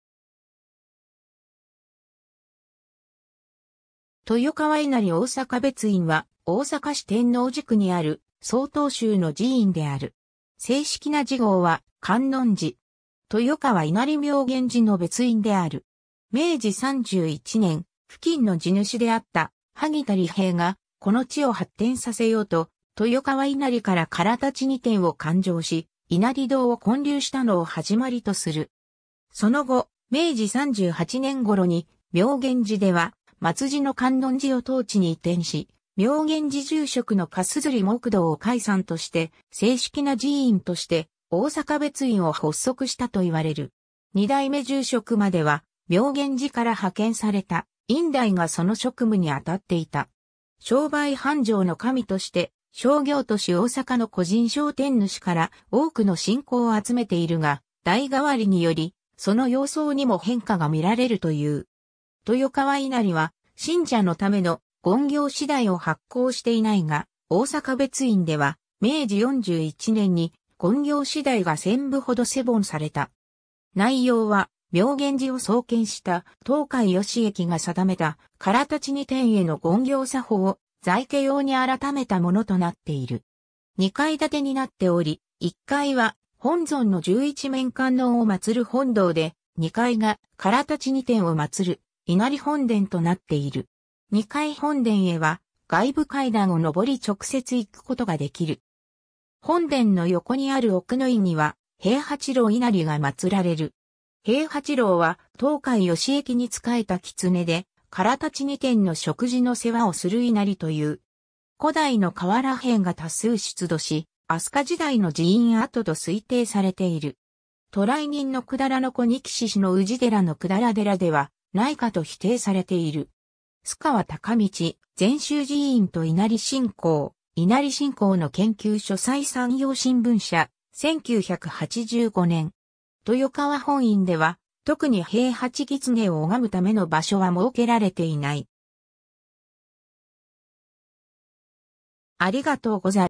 豊川稲荷大阪別院
豊川稲荷大阪別院（とよかわいなりおおさかべついん）は、大阪市天王寺区にある曹洞宗の寺院である。
正式な寺号は観音寺。
 豊川稲荷 妙厳寺（愛知県豊川市）の別院である。
 明治31年（1898年）、付近の地主であった萩田利平が、この地を発展させようと、豊川稲荷から吒枳尼天を勧請し、稲荷堂を建立したのを始まりとする。
 その後、明治38年頃に、妙厳寺では末寺の観音寺を当地に移転し、妙厳寺住職の嘉硯黙童を開山として、正式な寺院として大阪別院を発足したといわれる。
2代目住職までは、妙厳寺から派遣された院代がその職務に当たっていた。
 商売繁盛の神として、商業都市大阪の個人商店主から多くの信仰を集めているが、代替わりにより、その様相にも変化が見られるという。
 豊川稲荷は信者のための「勤行次第」（お経本）を発行していないが、大阪別院では、明治41年（1908年）に勤行次第が一千部ほど施本された。
内容は、妙厳寺を創建した東海義易が定めた吒枳尼天への勤行作法を、在家用に改めたものとなっている（下記リンク先参照）。
 2階建てになっており、1階は本尊の十一面観音を祀る本堂で、2階が吒枳尼天を祀る稲荷本殿となっている。
2階本殿へは外部階段を上り直接行くことができる（下記リンク先参照）。
 本殿の横にある奥の院には、平八郎稲荷が祀られる。
平八郎は東海義易に仕えた狐で、吒枳尼天の食事の世話をする稲荷という。
 古代の瓦片が多数出土し、飛鳥時代の寺院跡と推定されている。
渡来人の百済王氏の氏寺の百済寺ではないかと比定されている。
 ^ 栖川隆道　「禅宗寺院と稲荷信仰」 『稲荷信仰の研究』所載　山陽新聞社、1985年。
 ^ 豊川本院では、特に平八狐を拝むための場所は設けられていない。